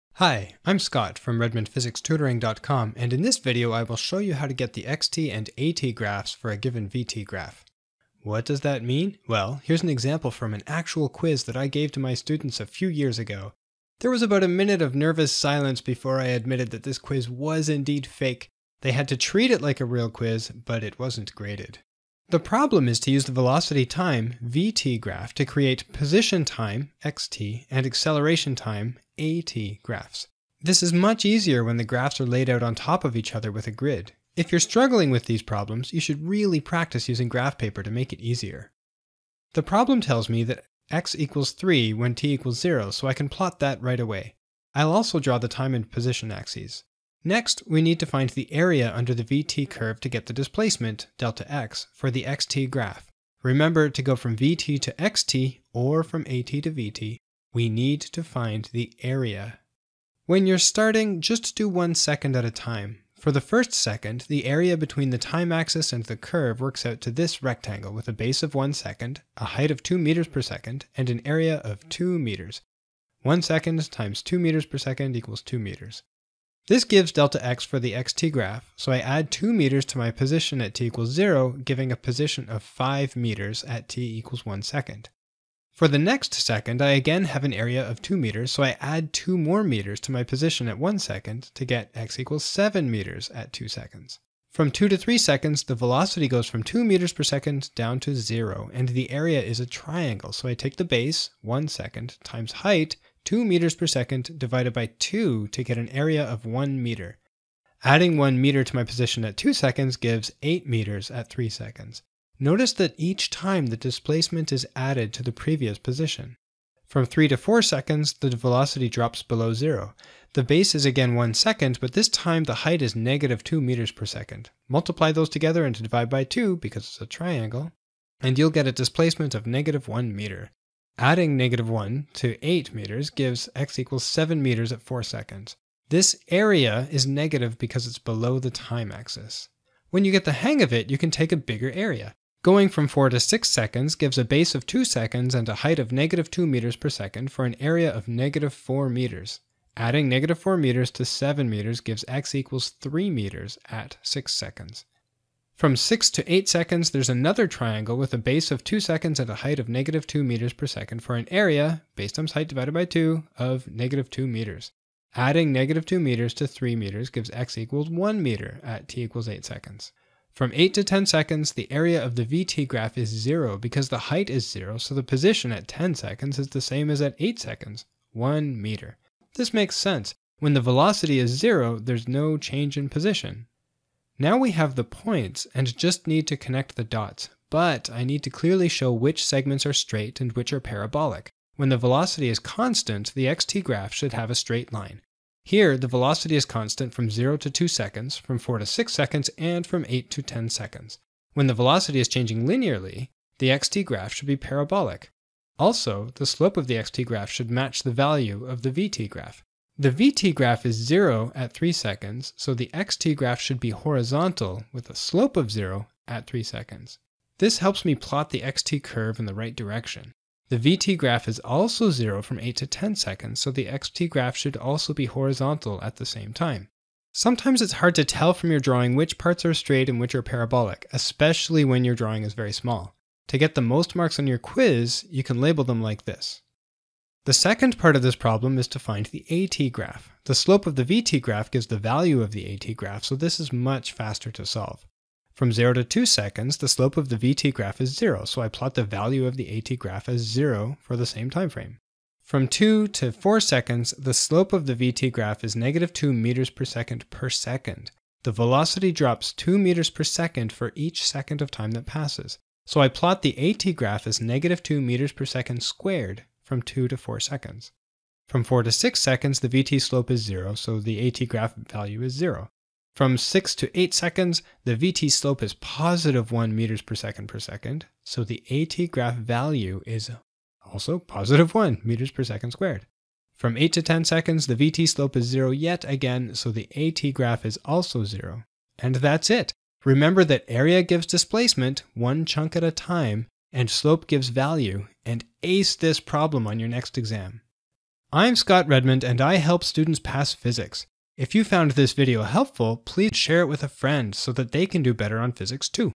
1D Kinematics Graphs: v-t to x-t and a-t
This video comes by special request from one of my tutoring students who appreciated how I explained the method for getting the position-time graph from the velocity-time graph.  This is a tricky skill and it's perfect for video because you can see how to work with each individual time segment.

Transcript:

Hi!  I'm Scott from RedmondPhysicsTutoring.com, and in this video I will show you how to get the x-t and a-t graphs for a given v-t graph.

What does that mean?  Well, here's an example from an actual quiz that I gave to my students a few years ago.  There was about a minute of nervous silence before I admitted that this quiz was fake; they had to treat it like a real quiz, but it wasn't graded.

This problem is to use the velocity-time (v-t) graph to create position-time (x-t) and acceleration-time (a-t) graphs.  This is much easier when the graphs are laid out on top of each other with a grid.  If you're struggling with these problems, you should really practice using graph paper to make it easier.

The problem tells me that the x = 3 when t = 0, so I can plot that right away.  I'll also draw the time and position axes.  Next, we need to find the area under the v-t curve to get the displacement, delta x, for the x-t graph.  Remember, to go from v-t to x-t, or from a-t to v-t, we need to find the area.

When you're starting, just do 1 second at a time.  For the first second, the area between the time axis and the curve works out to this rectangle with a base of 1 s, a height of 2 m/s, and an area of 2 m. 1 s times 2 m/s equals 2 m.  This gives delta x for the x-t graph, so I add 2 m to my position at t=0, giving a position of 5 m at t=1s.

For the next second, I again have an area of 2 m, so I add 2 m to my position at 1s to get x=7m at 2s.

From 2 to 3 seconds, the velocity goes from 2m/s down to zero, and the area is a triangle so I take base (1s) times height (2 m/s) divided by 2 to get an area of 1m.  Adding 1m to my position at 2s gives x=8m at 3s.  Notice that each time the displacement is added to the previous position.

From 3 to 4 seconds, the velocity drops below zero.  The base is again 1 s, but this time the height is -2m/s; multiply those together and divide by 2 (because it's a triangle) and you'll get a displacement of -1m.  Adding -1m to 8m gives x=7m at 4s.  This area is negative because it's below the time axis.

When you get the hang of it, you can take a bigger area.  Going from 4 to 6 s gives a base of 2 s and a height of -2m/s for an area of -4 m.  Adding -4m to 7m gives x=3m at 6s.

From 6 to 8 s there's another triangle with a base of 2 s and a height of -2 m/s for an area (base times height divided by 2) of -2 m.  Adding -2m to 3m gives x=1m at t=8s.

From 8 to 10 s, the area of the v-t graph is zero, because the height is zero, so the position at 10s is the same as at 8s: 1m. This makes sense; when the velocity is zero there is no change in position.

Now we have the points, and just need to connect the dots.  BUT I need to clearly show which segments are straight and which are parabolic.  When the velocity is constant, the x-t graph should have a straight line.  Here, the velocity is constant from 0 to 2s, from 4 to 6s, and from 8 to 10s.  When the velocity is changing linearly, the x-t graph should be parabolic.  Also, the slope of the x-t graph should match the value of the v-t graph.  The v-t graph is zero at 3s so the x-t graph should be horizontal at 3s.  This helps me plot the x-t curve in the right direction.  The v-t graph is also zero from 8-10s, so the x-t graph should also be horizontal from 8-10s.

Sometimes it's hard to tell from your drawing which parts are straight and which are parabolic.  To get the most marks on your quiz, you can label them like this.

The second part of this problem is to find the a-t graph.  The slope of the v-t graph gives the value of the a-t graph, so this is much faster to solve.

From 0 to 2s, the slope of the v-t graph is zero, so I plot the value of the a-t graph as zero for the same time frame.

From 2 -o 4s, the slope of the v-t graph is -2m/s per second; the velocity drops 2m/s for each second of time that passes.  So I plot the a-t graph as -2m/s^2 from 2 to 4s.

From 4 to 6s, the v-t slope is zero, so the a-t graph value is zero.

From 6 to 8s, the v-t slope is positive 1 m/s per second, so the a-t graph value is 1 m/s^2.

From 8 to 10s, the v-t slope is zero yet again, so the a-t graph is also zero.

That's it!  Remember that area gives displacement, one chunk at a time, and slope gives value, and ace this problem on your next exam!

I'm Scott Redmond and I help students pass physics.  If you found this video helpful, please share it with a friend so they can do better on physics too!